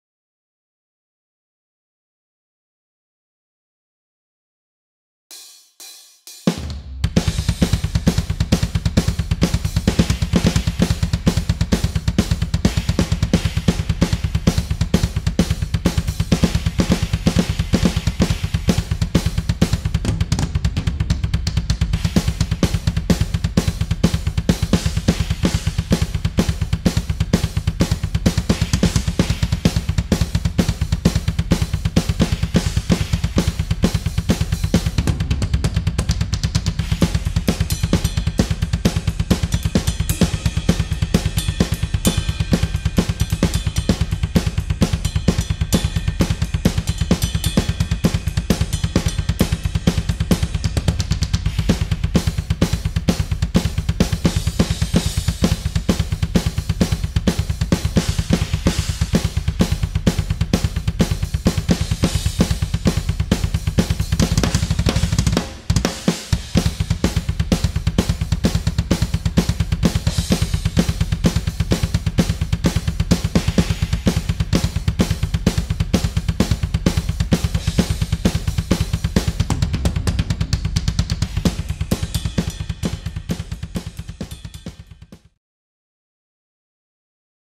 BATERIA YAMAHA DTX 500K + ADDCITIVE DRUMS 2 + REAPER
Testando o novo KIT de bateria

Yamaha DTX + Michael DDM + Addictive Drums 2 + Reaper

Ficou bacana para tirar umas músicas com o duplo :)